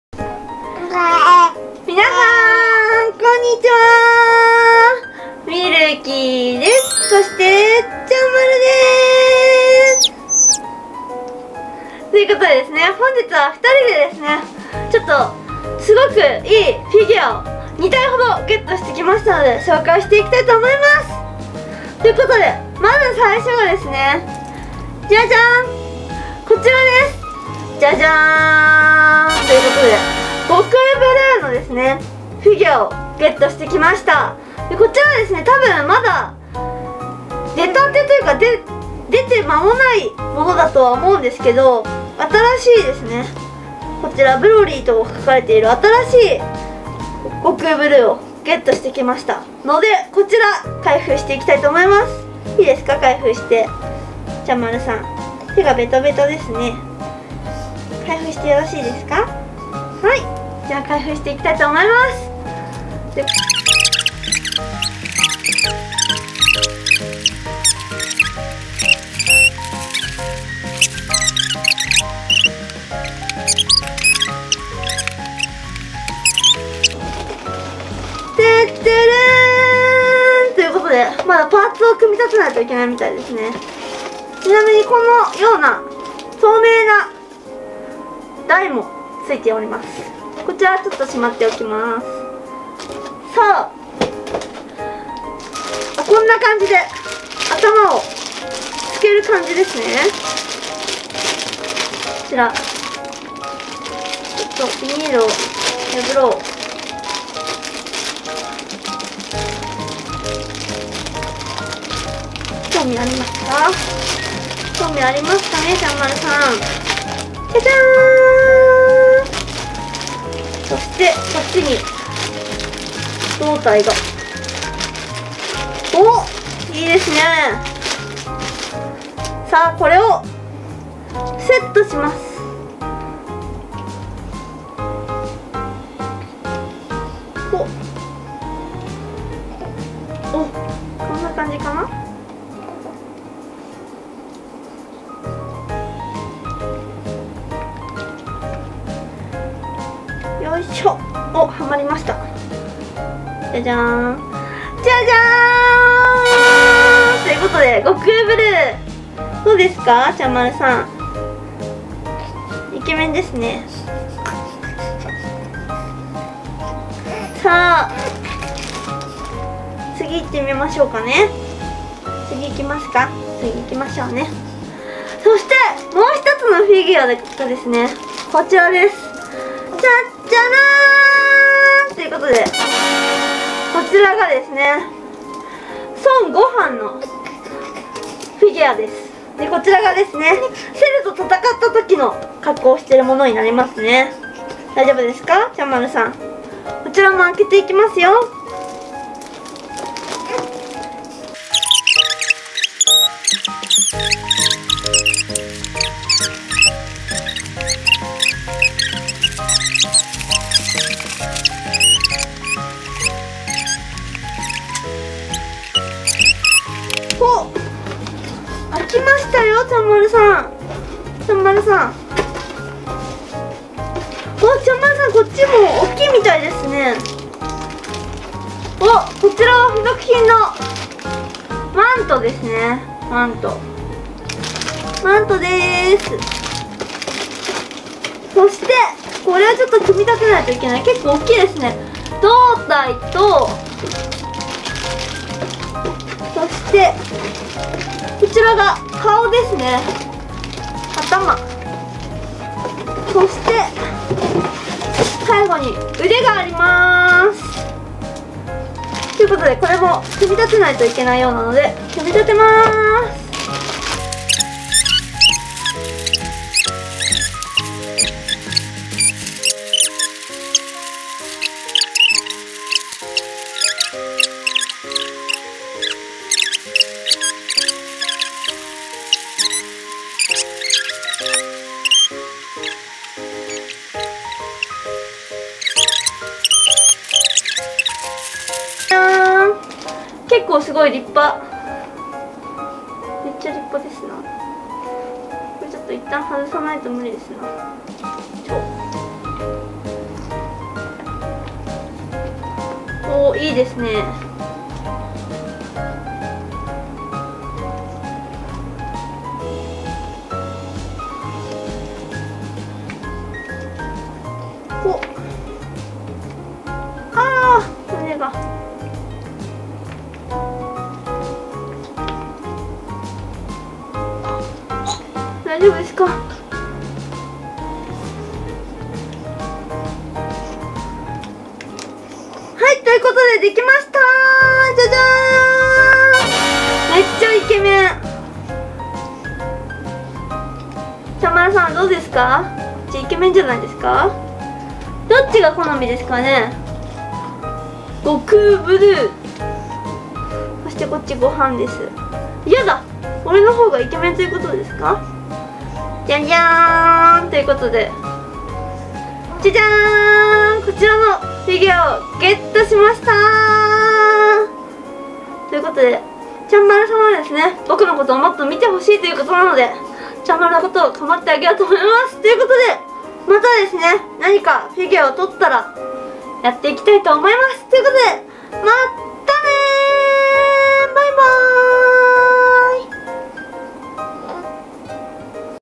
【クレーンゲーム】ドラゴンボールフィギュアゲット♪
ドラゴンボールの悟空ブルーと悟飯のフィギュアをゲットしました☆
どっちもイケメンですごくかっこいいです♪
見かけた方は是非狙ってみてくださいね!(^^)!